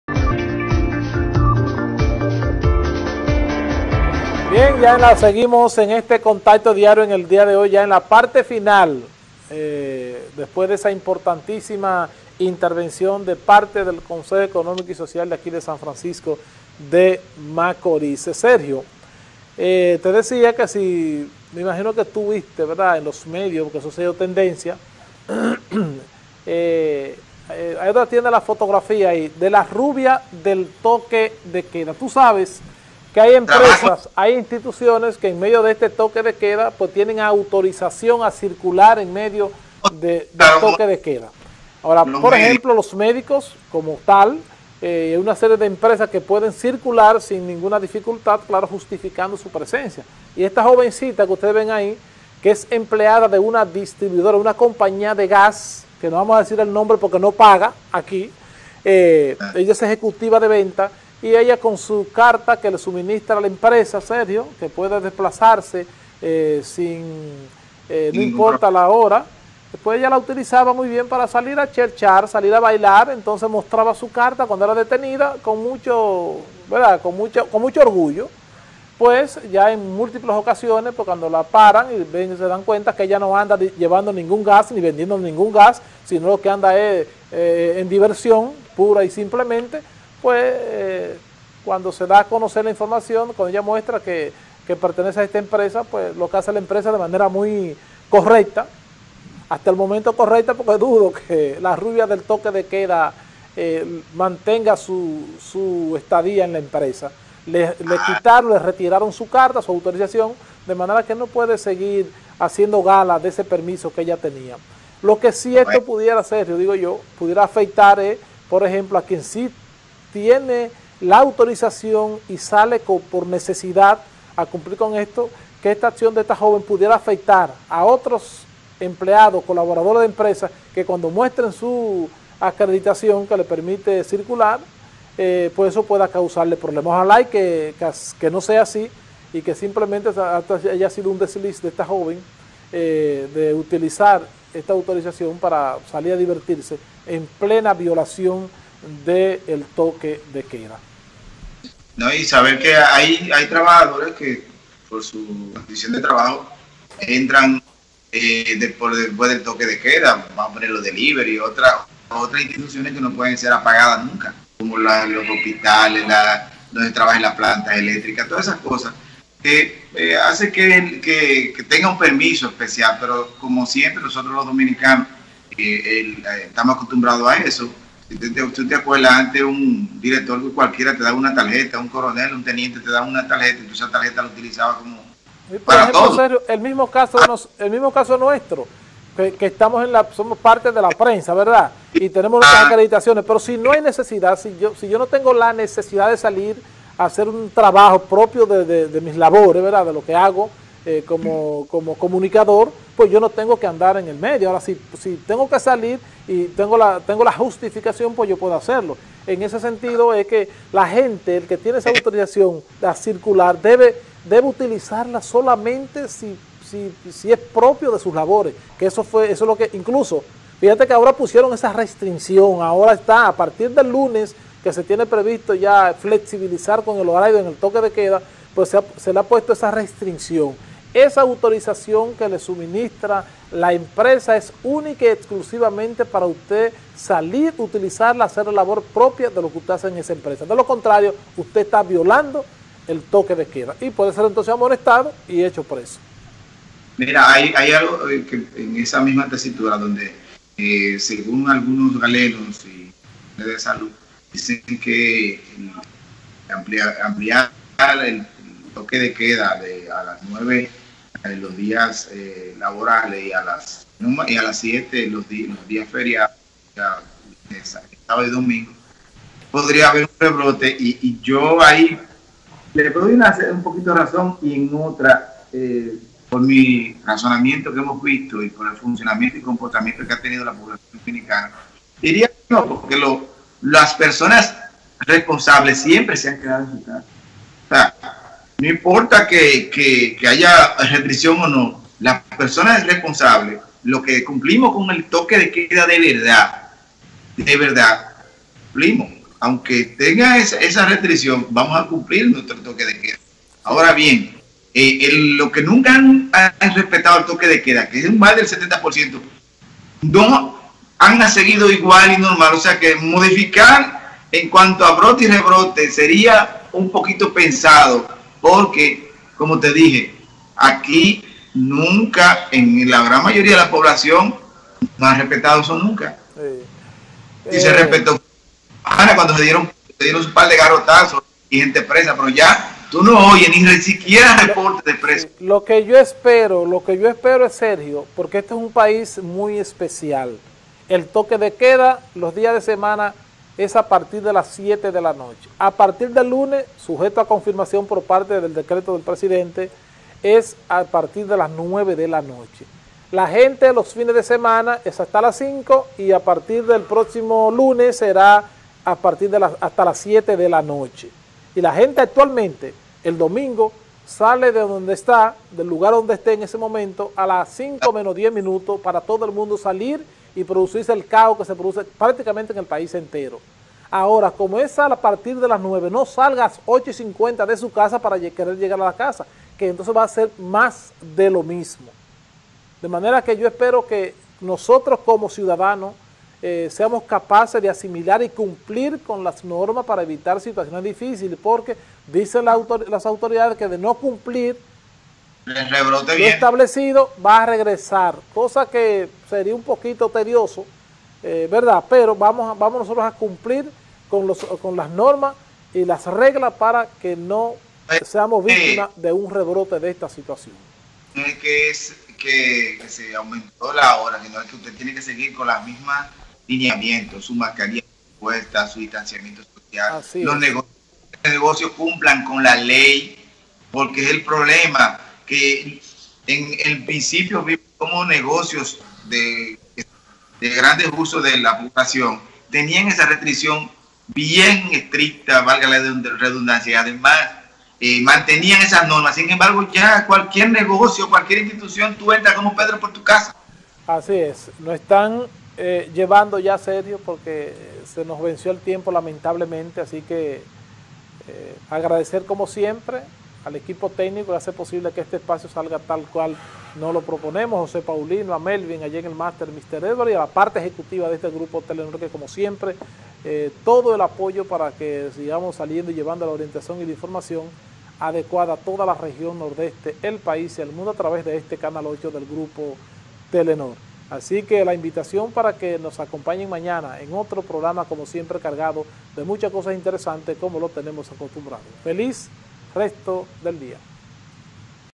Los ciudadanos deben asimilar y cumplir las medidas si no queremos un rebrote del Covid-19
Los ciudadanos deben asimilar y cumplir las medidas si no queremos un rebrote del Covid-19 dice Victor Puntiel